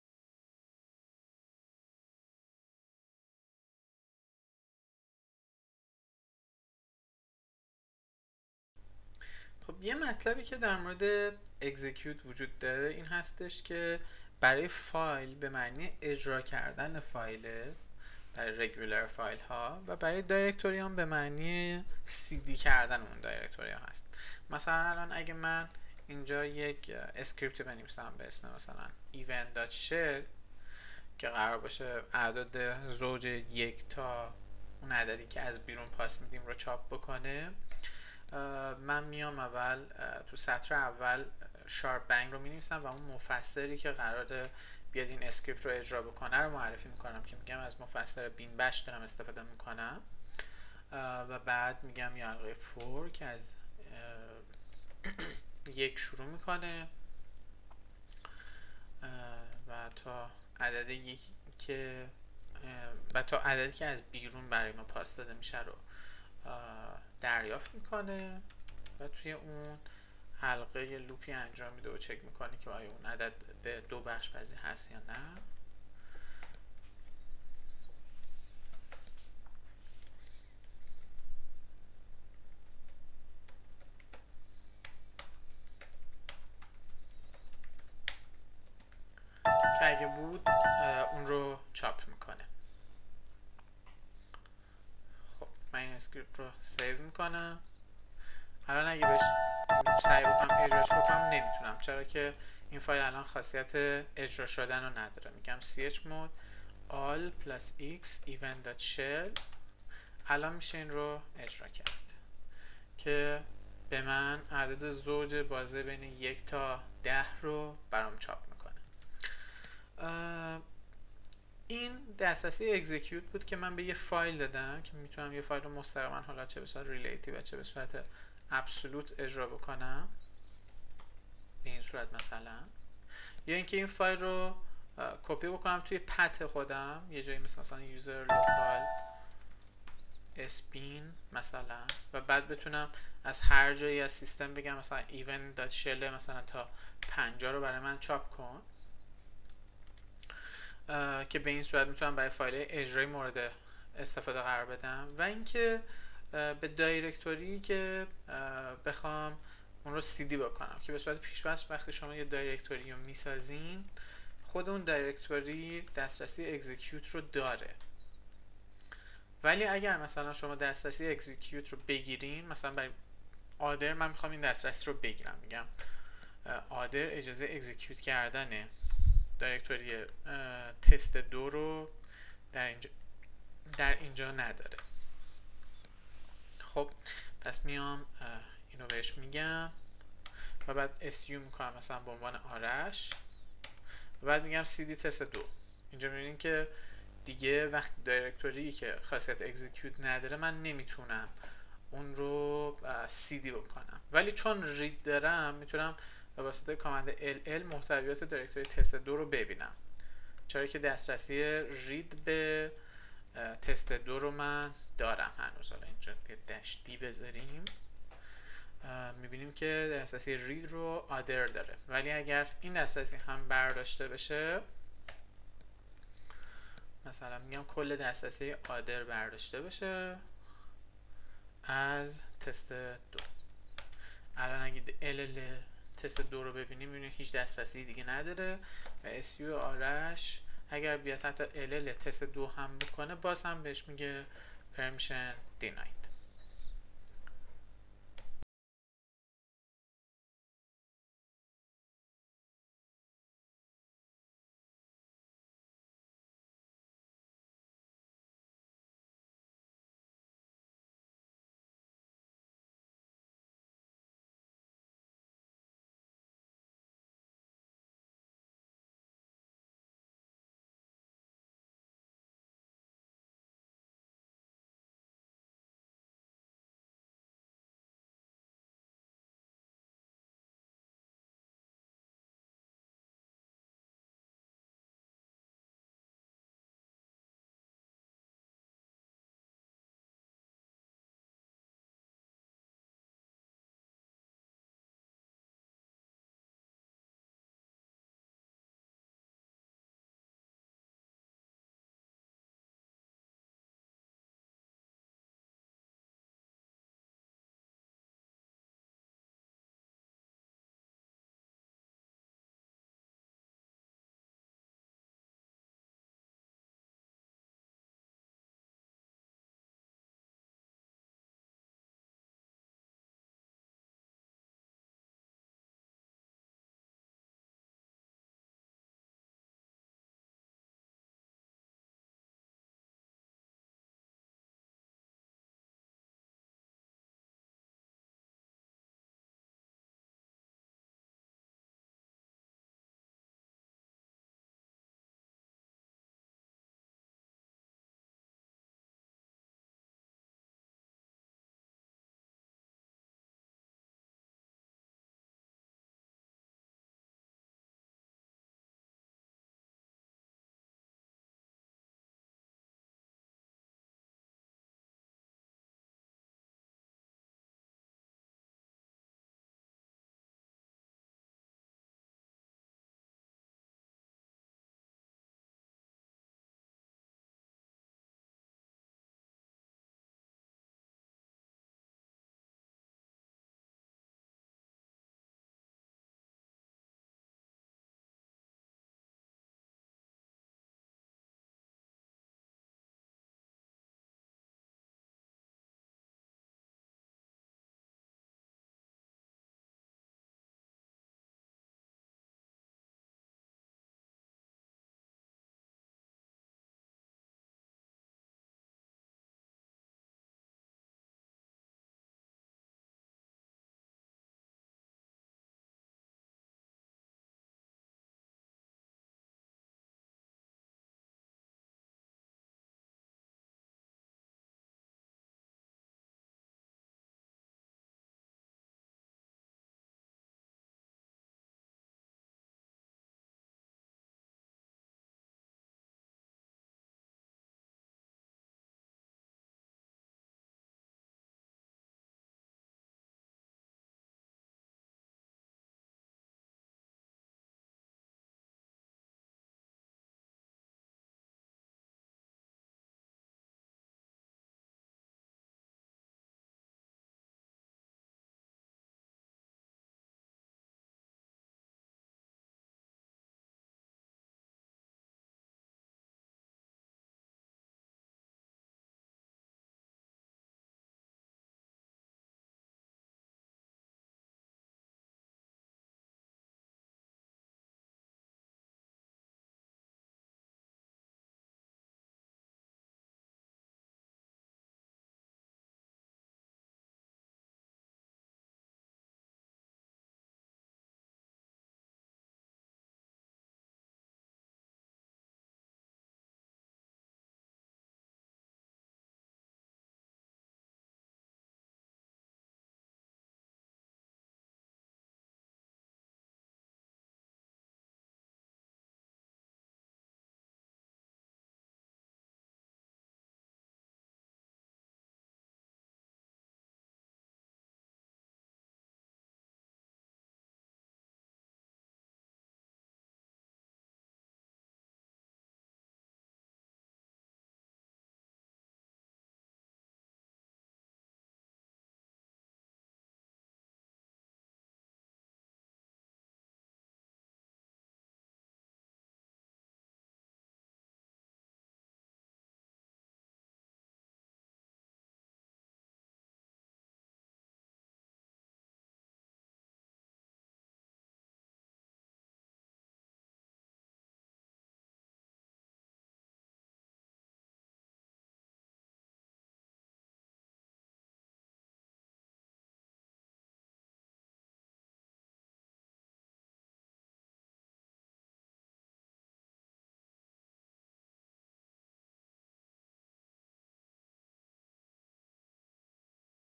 لینوکس مقدماتی - درس 14: مدیریت دسترسی ها – بخش 2
این بخش ادامه مدیریت دسترسی و اعطا دسترسی ها است، که در خصوص نحوه ارائه مجوزهای پایه ای در لینوکس بر اساس مطالب گفته شده در بخش بعدی صحبت می‌کند.